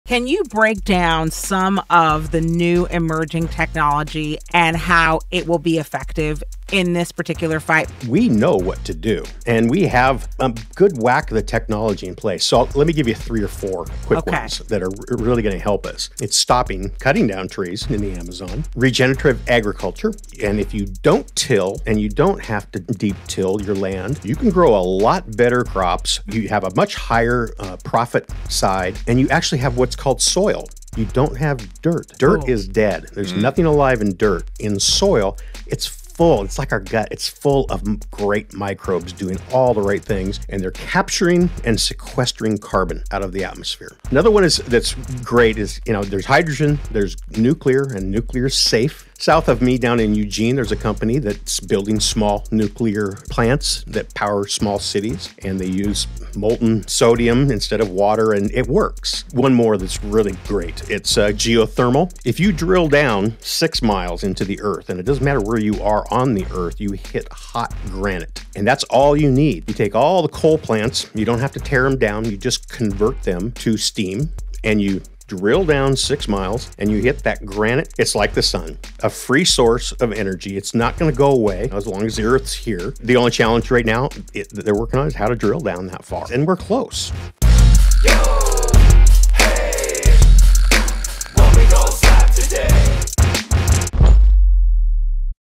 Emerging Technology - SLAP The Power - Climate Change
The emerging tech rabbit hole is inspiring and filled with hope! So many people are making amazing strides to aid in our fight to save our planet!